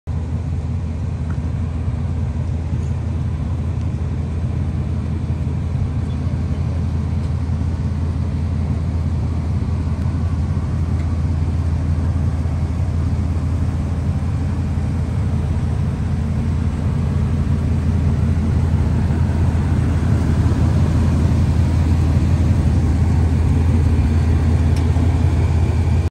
Ford SVT Lightning or OBS Chevy?
Just a quick video of my truck and my friends OBS.
Which one would you take?